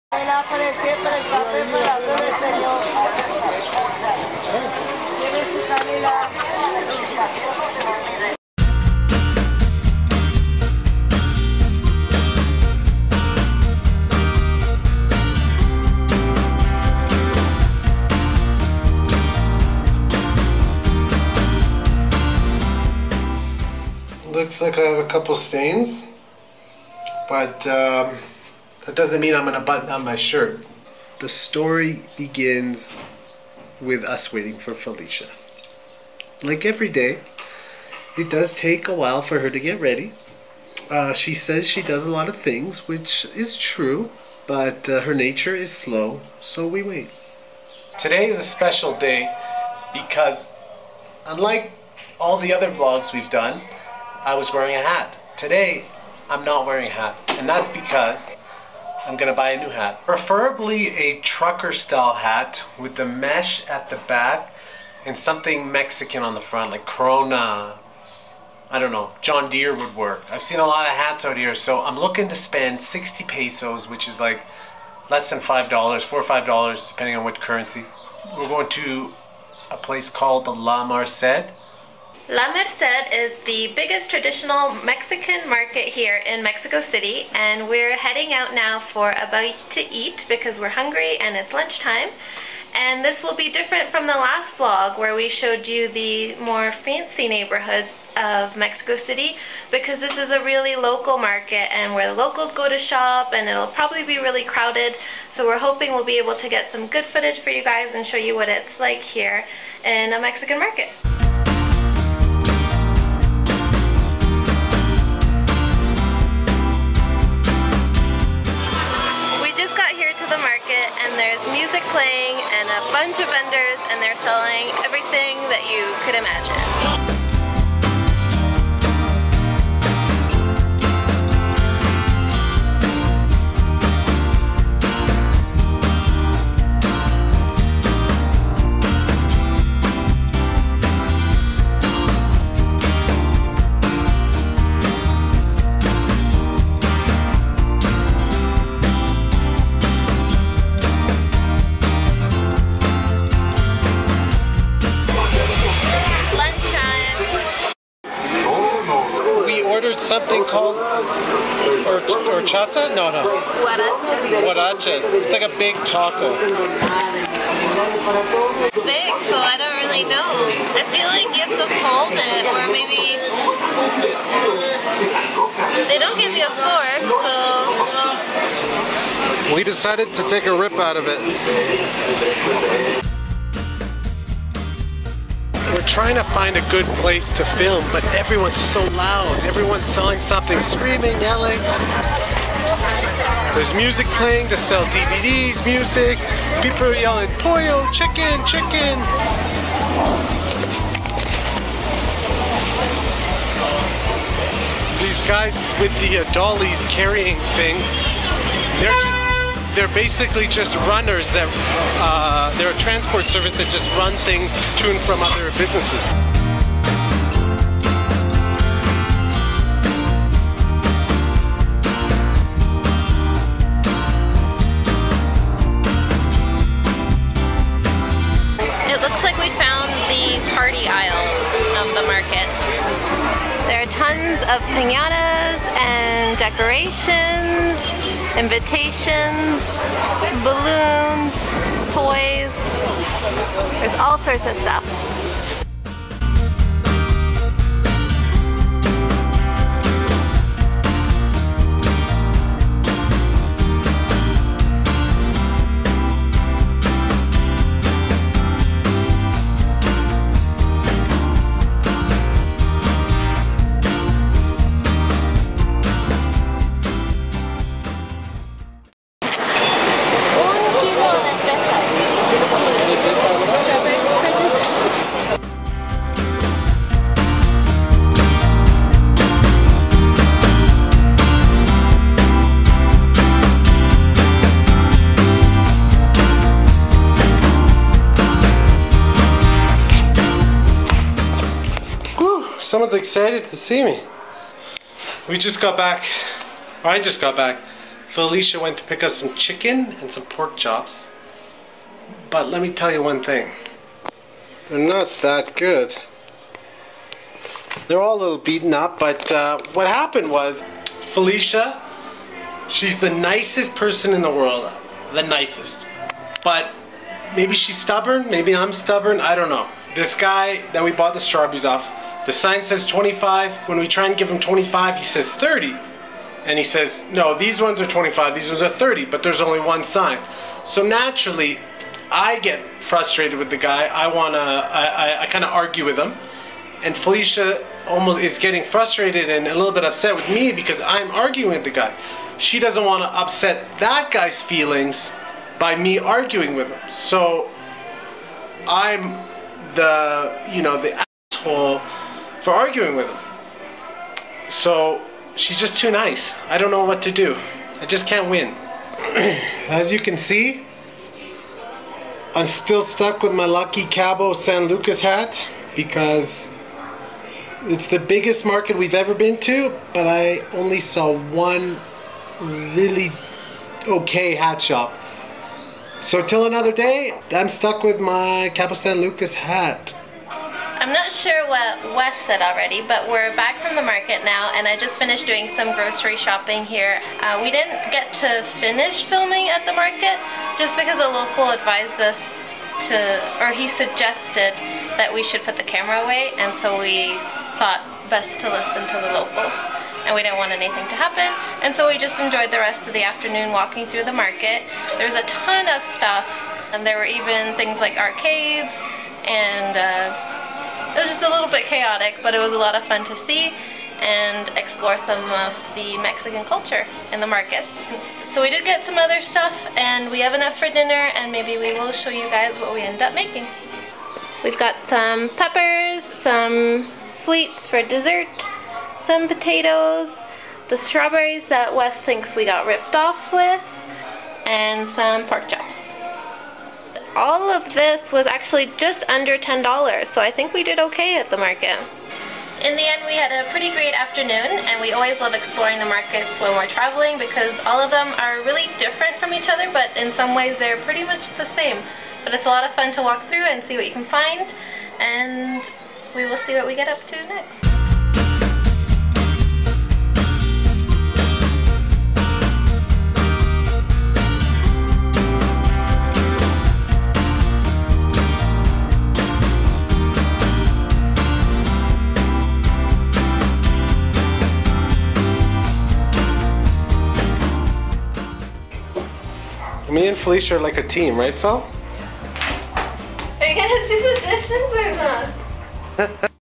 Shopping at MEXICO CITY'S BIGGEST Traditional Market
In this Vlog we travel to La Merced Mexico City: the biggest traditional market in Mexico City. We look for a hat, some dinner and we find out half way through that it might not be the safest place to bring a camera.

NEW TO AIRBNB? Get a free credit towards your first stay when you sign up through this